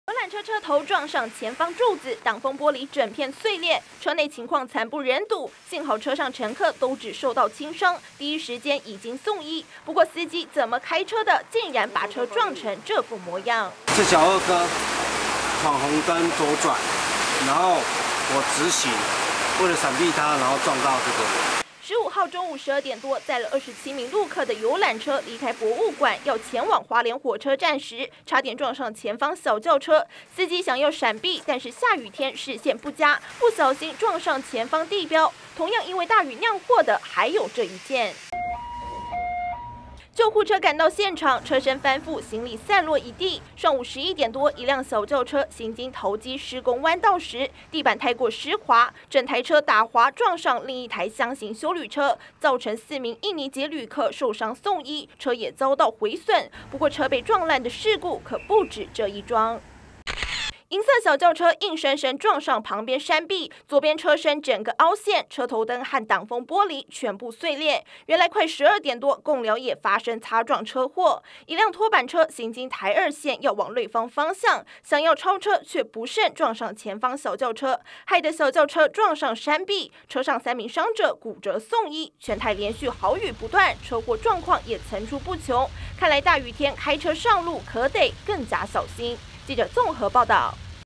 20171015中天新聞 全台豪大雨不斷！ 各地車禍「撞」況連連
全台豪雨不斷，今天各地不停有車禍發生，幾乎都是因為天雨路滑，加上視線不佳才會釀禍，像是15號早上，花蓮就有一輛載著陸客的遊覽車，閃避不及撞上地標，還有桃園機場的施工彎道，也有轎車打滑撞翻車的意外，另外貢寮同樣也有車輛，擦撞後撞上山壁，而且都有造成人員受傷。